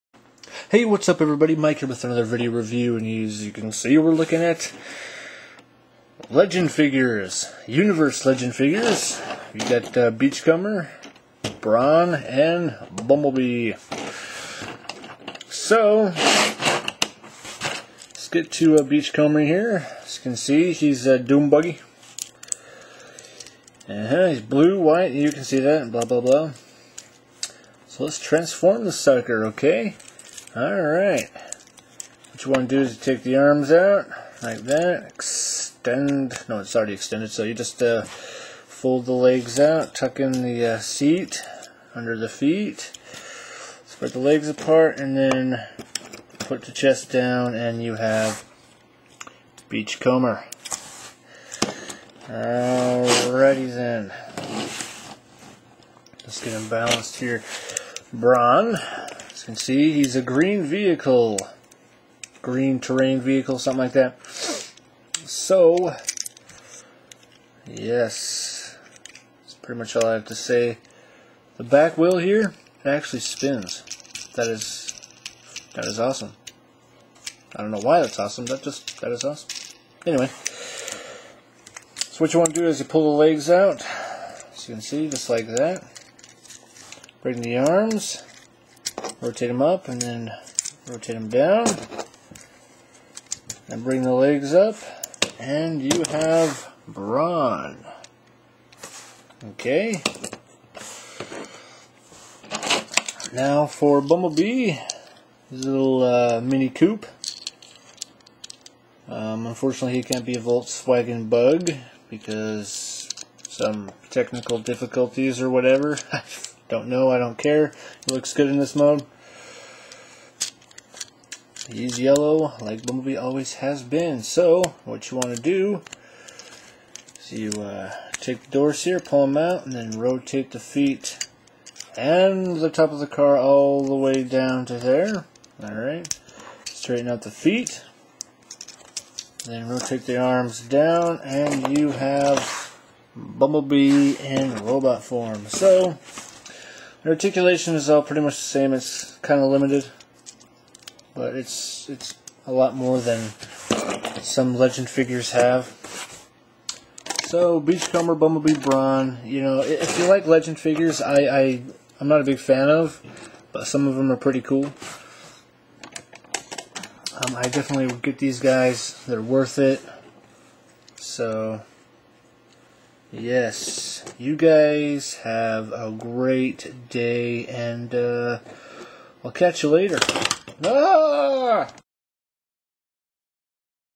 Transformers Legends Bumblebee Beach Comber Brawn Review
Note:  the green screen turned brawn blue, he is actually green.